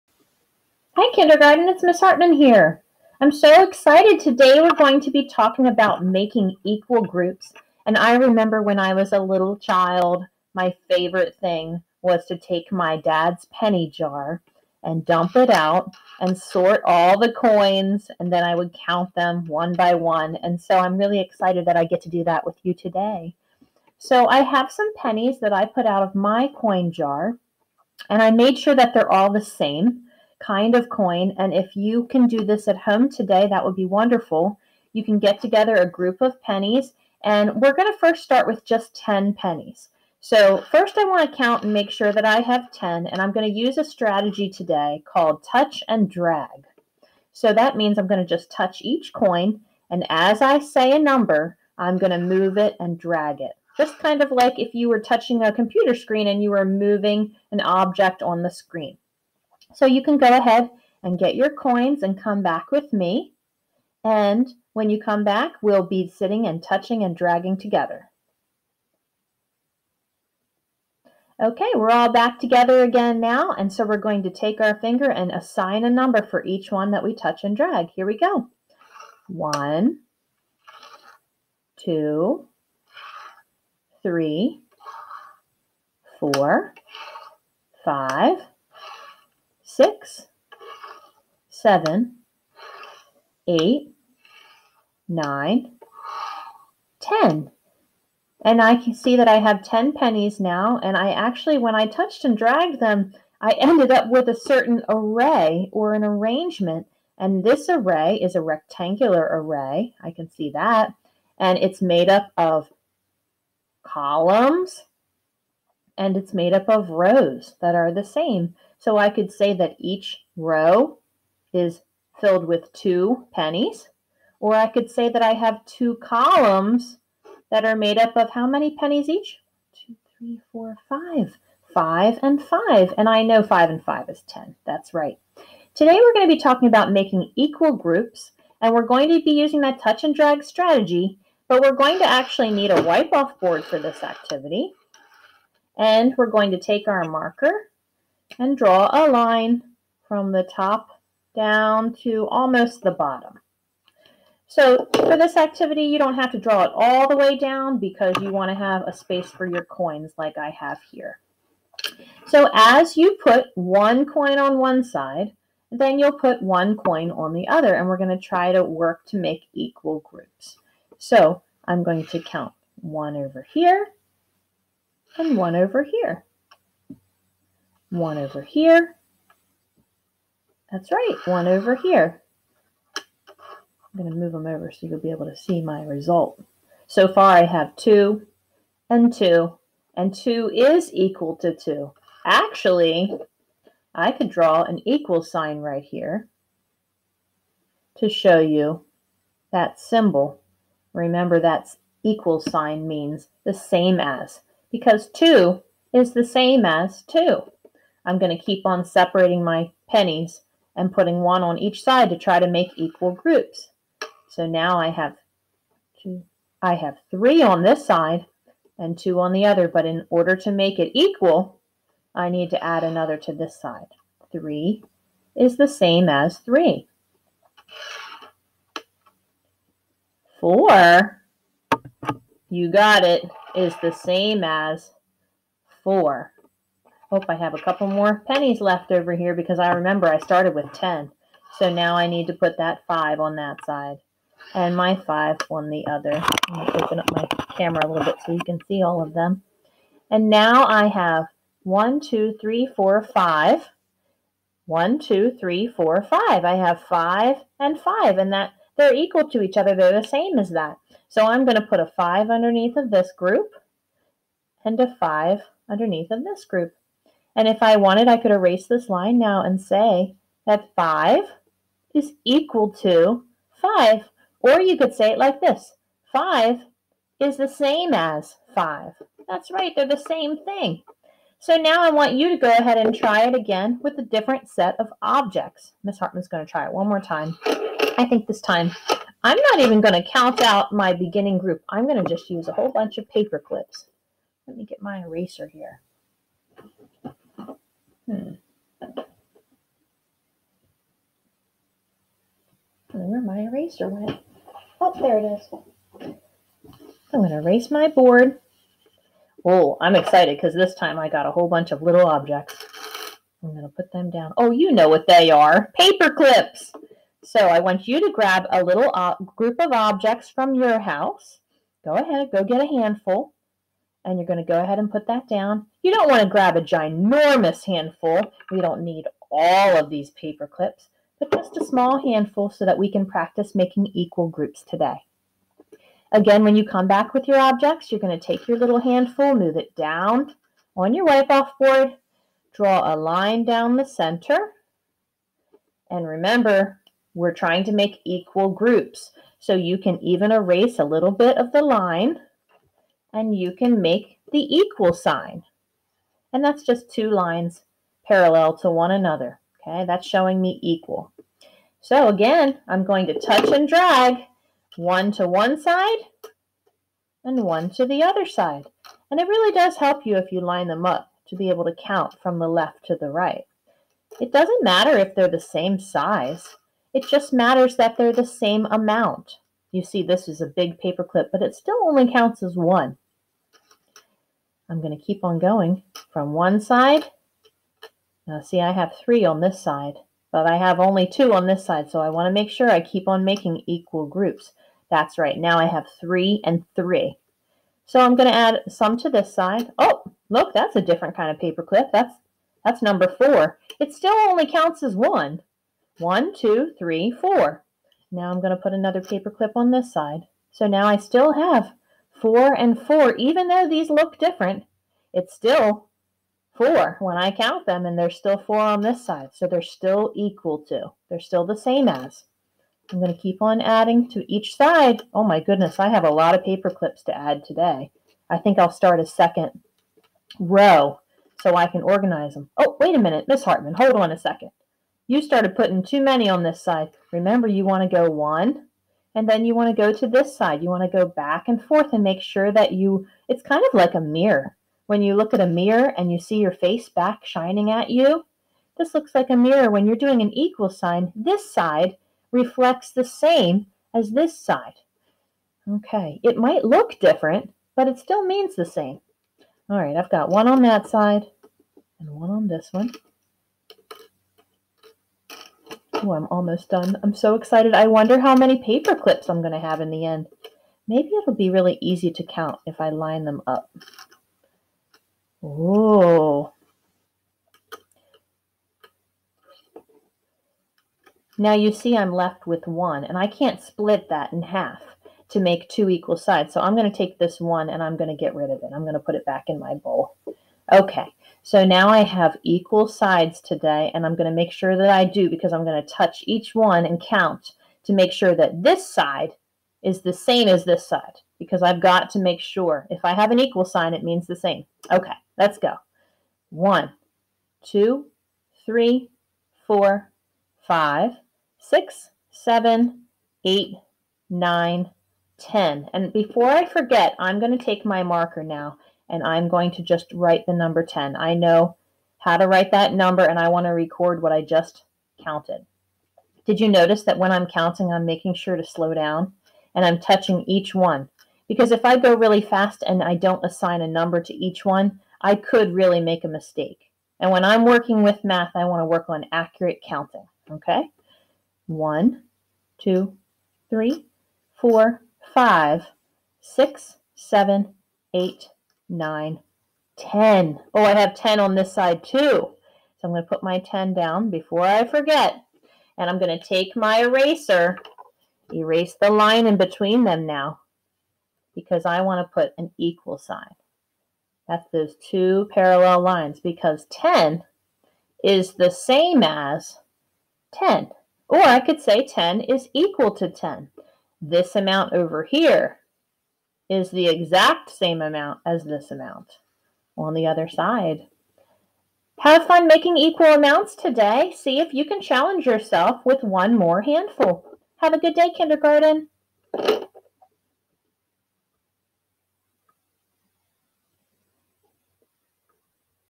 Mrs. Hartman's Making Equal Groups: Teach It
Use touch and drag strategies to organize a handful of small objects to create equal groups and utilize the equal symbol.